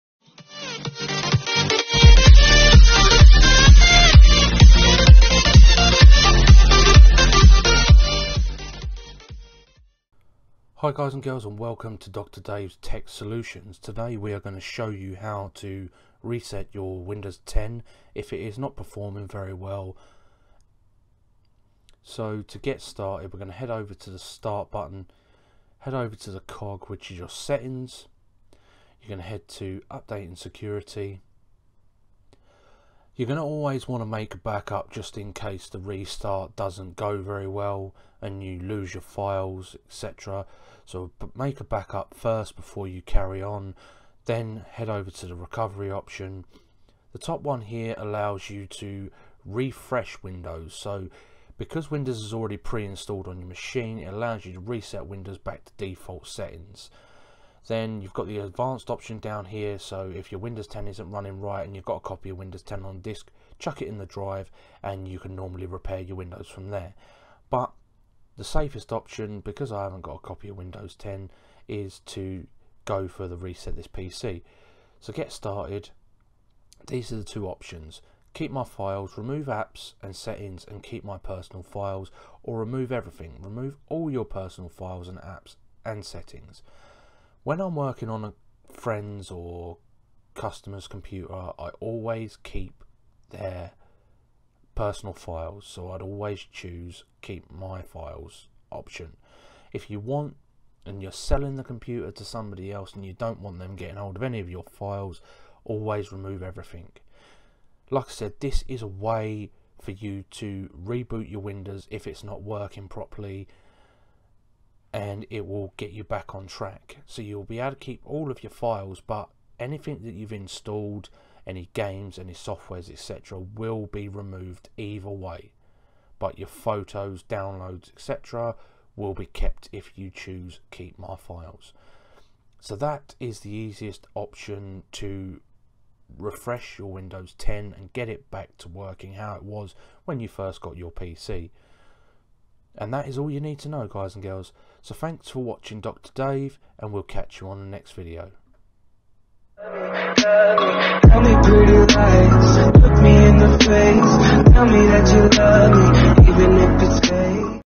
WINDOWS 10 RESET WITH OUT DISK OR USB (EASY)
in this video i will show you how to reset your windows 10 back to factory setting with out having to use a disk or usb

this will help if your pc is not preforming well of you have some errors that you can not fix

this is an easy guide for beginners so hope this helps and goodluck